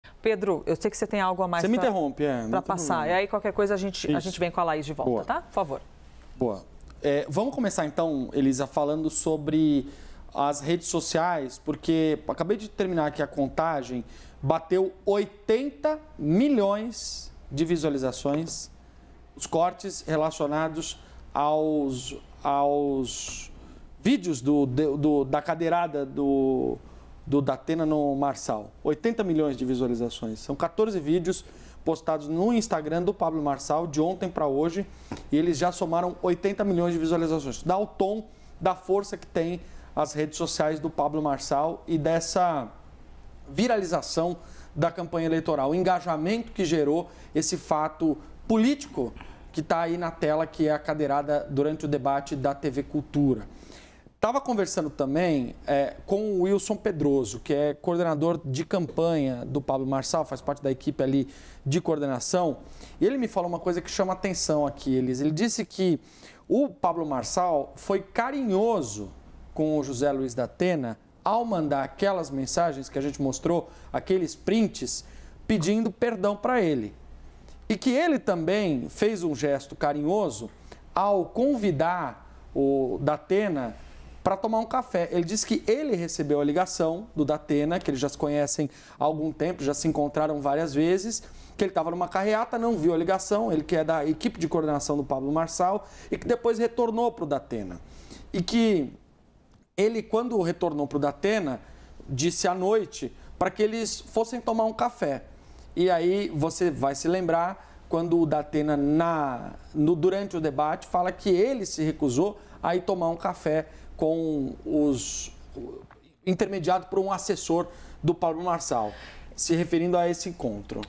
Campanha de Marçal diz que candidato foi “carinhoso” com Datena ao pedir desculpas | LIVE CNN
A campanha de Pablo Marçal afirmou à CNN que o candidato à prefeitura de São Paulo pelo PRTB foi “carinhoso” com o adversário José Luiz Datena (PSDB) ao enviar uma mensagem pedindo desculpas a Datena e convidá-lo para tomar um café. As declarações foram feitas antes do debate da TV Cultura em que Datena agrediu Marçal com uma cadeira.